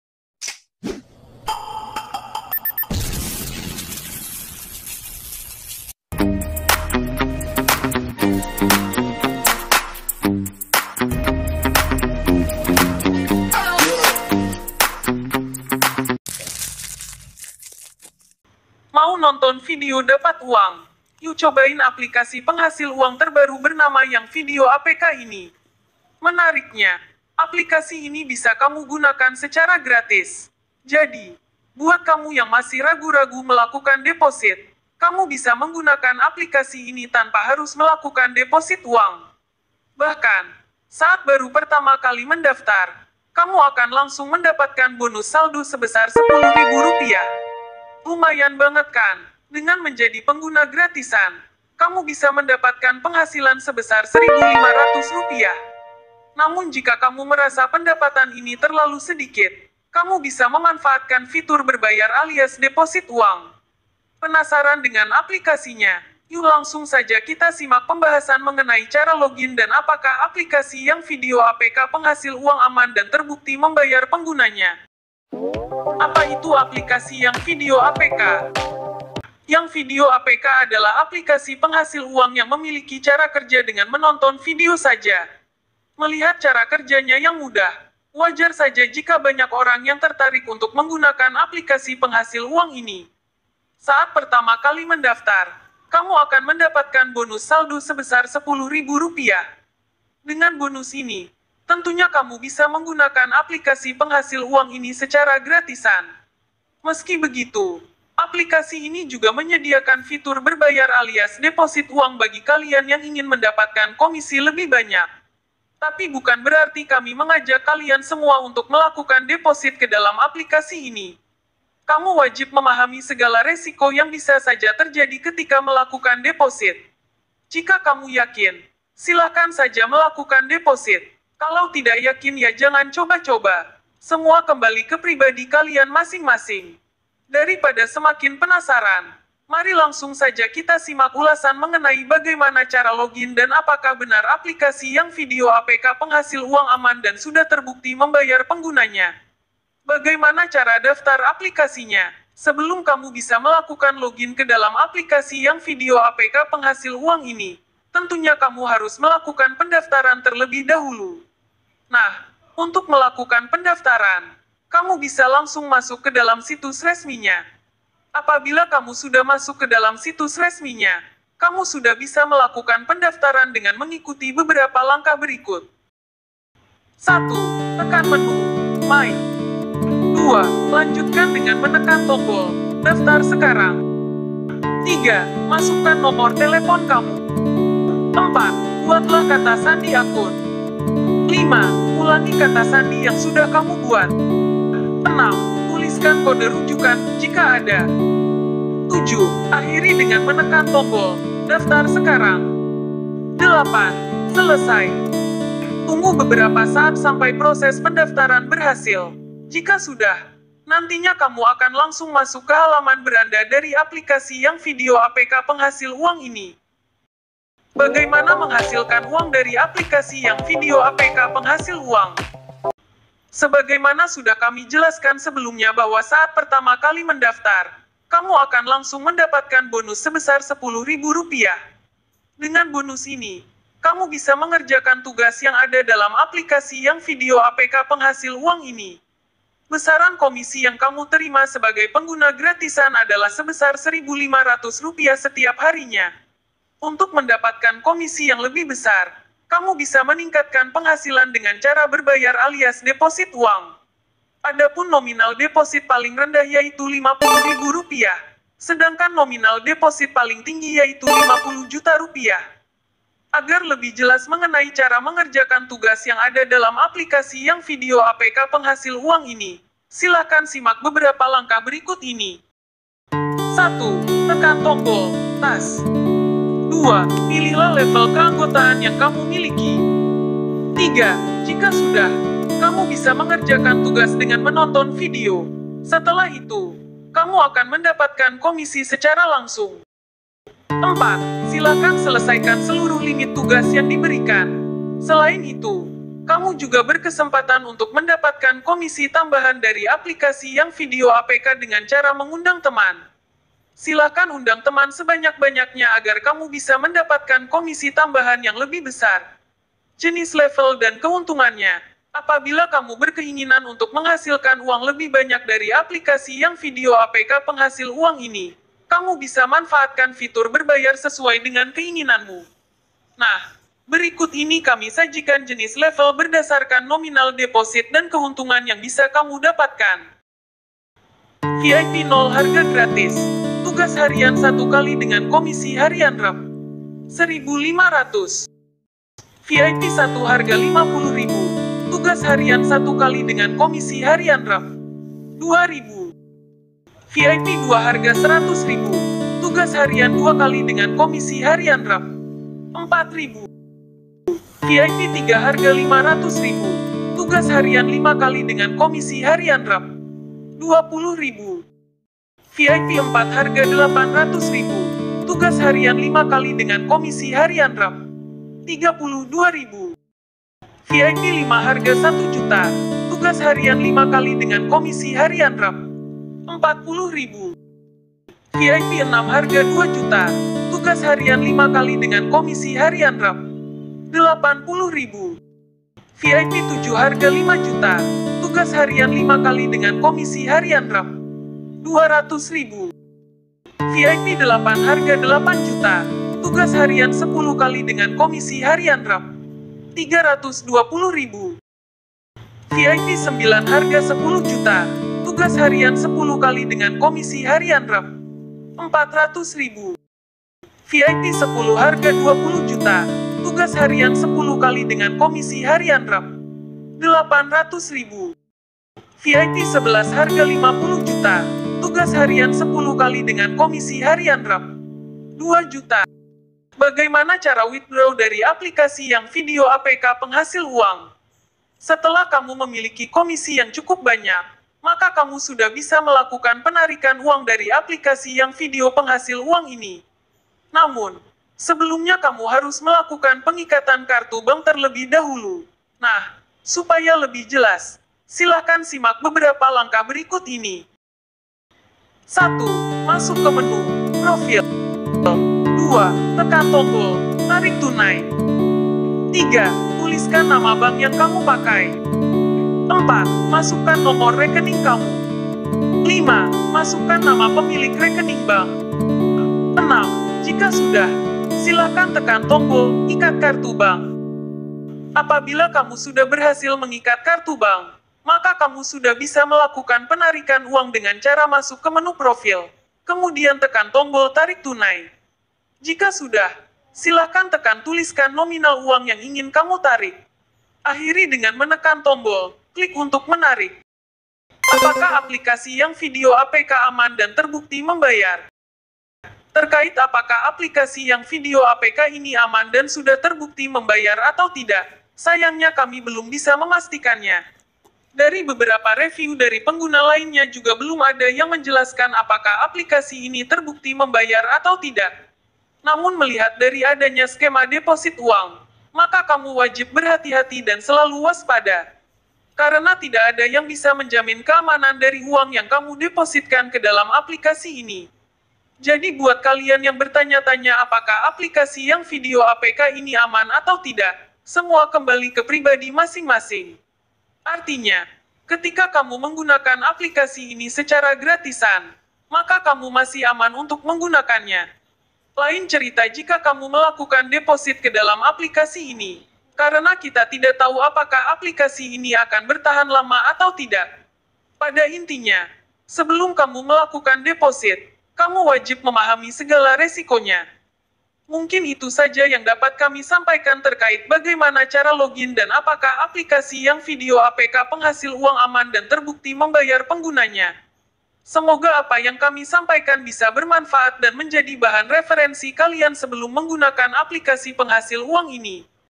Aplikasi Young Video Penghasil Uang, Apakah Aman
mau nonton video dapat uang? Yuk cobain aplikasi penghasil uang terbaru bernama young video apk ini.

Menariknya, aplikasi ini bisa kamu gunakan secara gratis. Jadi, buat kamu yang masih ragu-ragu melakukan deposit, kamu bisa menggunakan aplikasi ini tanpa harus melakukan deposit uang.

Bahkan, saat baru pertama kali mendaftar, kamu akan langsung mendapatkan bonus saldo sebesar 10.000 rupiah. Lumayan banget kan?

Dengan menjadi pengguna gratisan, kamu bisa mendapatkan penghasilan sebesar 1.500 rupiah. Namun jika kamu merasa pendapatan ini terlalu sedikit, kamu bisa memanfaatkan fitur berbayar alias deposit uang.

Penasaran dengan aplikasinya? Yuk langsung saja kita simak pembahasan mengenai cara login dan apakah aplikasi young video apk penghasil uang aman dan terbukti membayar penggunanya.

Apa Itu Aplikasi Young Video Apk?

Young video apk adalah aplikasi penghasil uang yang memiliki cara kerja dengan menonton video saja. Melihat cara kerjanya yang mudah, wajar saja jika banyak orang yang tertarik untuk menggunakan aplikasi penghasil uang ini.

Saat pertama kali mendaftar, kamu akan mendapatkan bonus saldo sebesar 10.000 rupiah. Dengan bonus ini, tentunya kamu bisa menggunakan aplikasi penghasil uang ini secara gratisan.

Meski begitu, aplikasi ini juga menyediakan fitur berbayar alias deposit uang bagi kalian yang ingin mendapatkan komisi lebih banyak. Tapi bukan berarti kami mengajak kalian semua untuk melakukan deposit ke dalam aplikasi ini.

Kamu wajib memahami segala resiko yang bisa saja terjadi ketika melakukan deposit. Jika kamu yakin, silahkan saja melakukan deposit. Kalau tidak yakin ya jangan coba-coba. Semua kembali ke pribadi kalian masing-masing.

Dari pada semakin penasaran, mari langsung saja kita simak ulasan mengenai bagaimana cara login dan apakah benar aplikasi young video apk penghasil uang aman dan sudah terbukti membayar penggunanya.

Bagaimana Cara Daftar Aplikasinya?

Sebelum kamu bisa melakukan login ke dalam aplikasi young video apk penghasil uang ini, tentunya kamu harus melakukan pendaftaran terlebih dahulu.

Nah, untuk melakukan pendaftaran, kamu bisa langsung masuk ke dalam situs resminya

Apabila kamu sudah masuk ke dalam situs resminya, kamu sudah bisa melakukan pendaftaran dengan mengikuti beberapa langkah berikut.